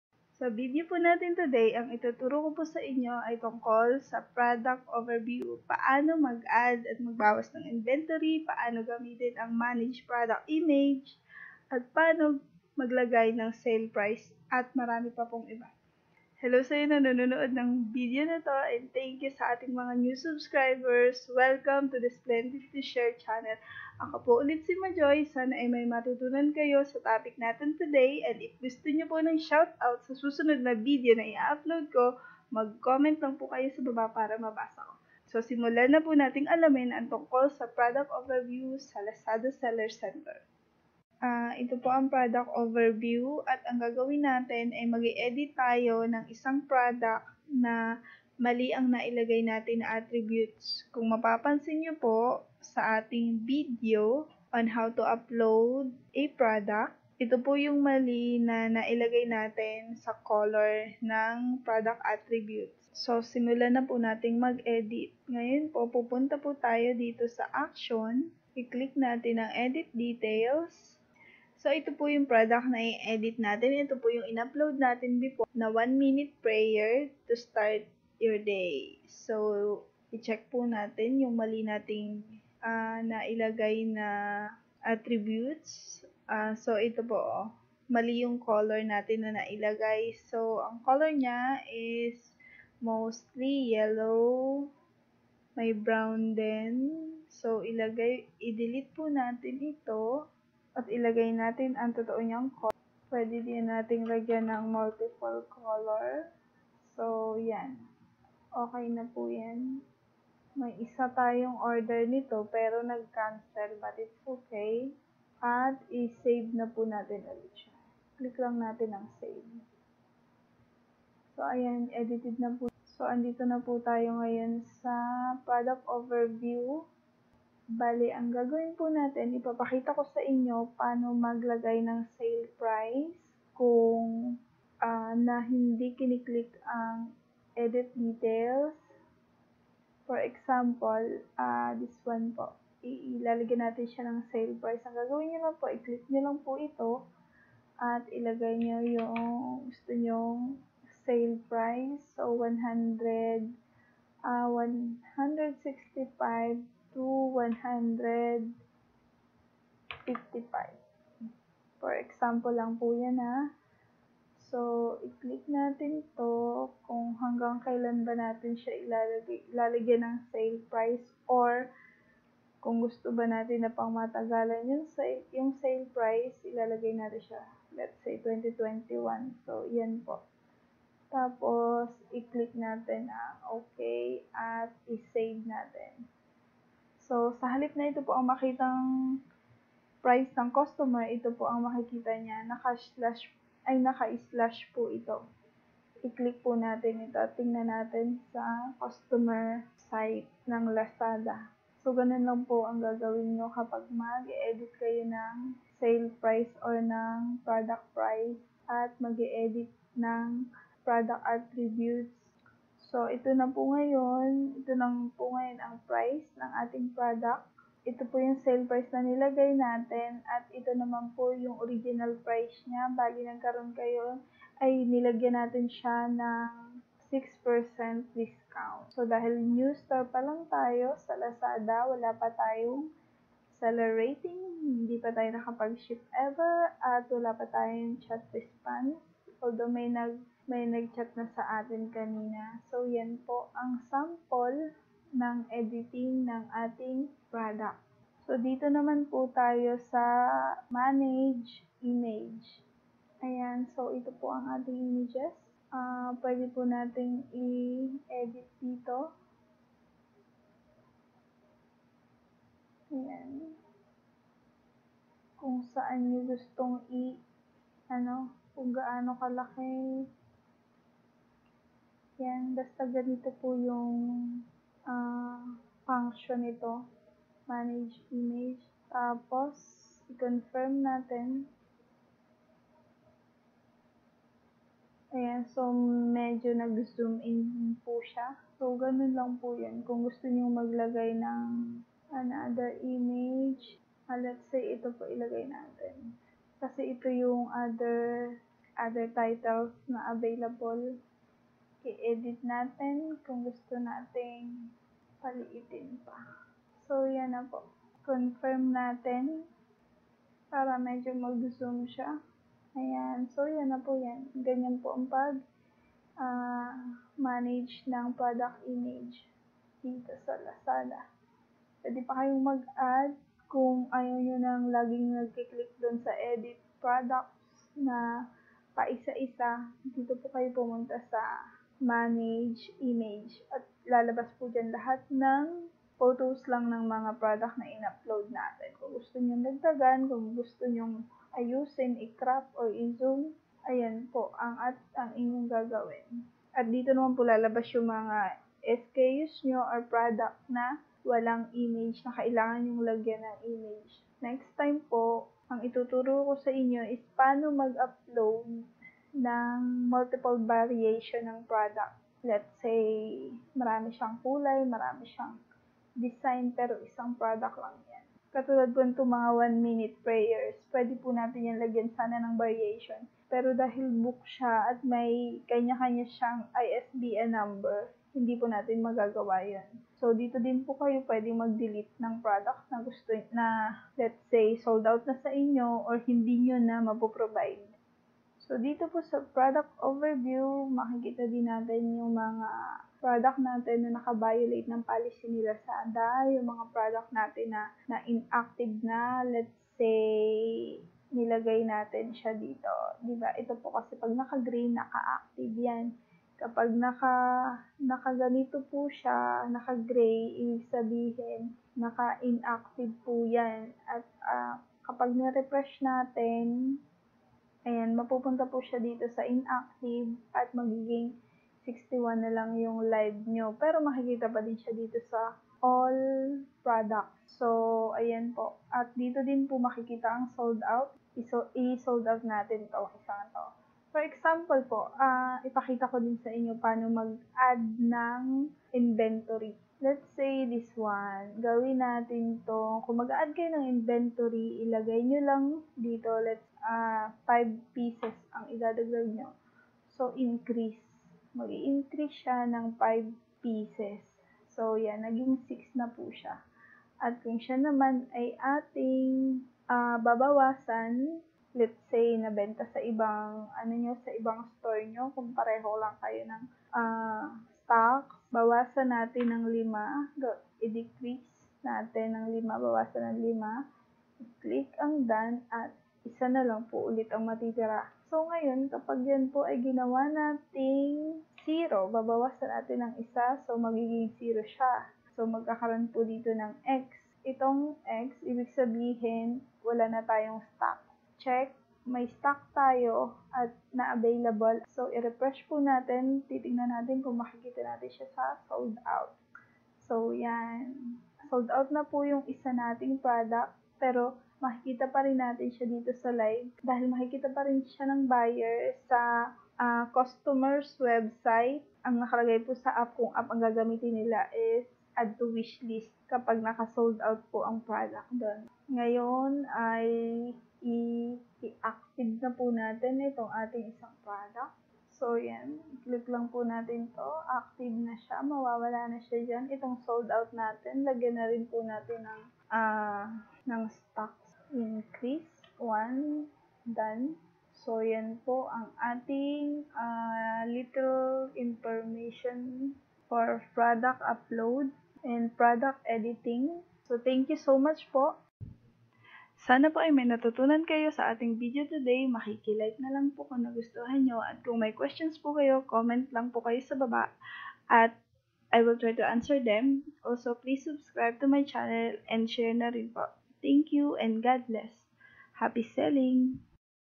PAANO MAG-LAGAY NG PRODUCT SALE PRICE, MAG-MANAGE NG PRODUCT IMAGE, INVENTORY SA LAZADA AT IBA PA
PAANO MAGLAGAY NG PRODUCT SALE PRICE, MAGMANAGE NG PRODUCT IMAGE, INVENTORY SA LAZADA AT IBA PA - FOR NEWBIE

Ang video na ito ay tungkol sa Product Overview sa Lazada seller center.
Nakadetalye kung paano mag-edit ng product, paano maglagay ng inventory, mag-ayos image ng product (shortcut) paano maglagay ng sale price, at marami pang iba.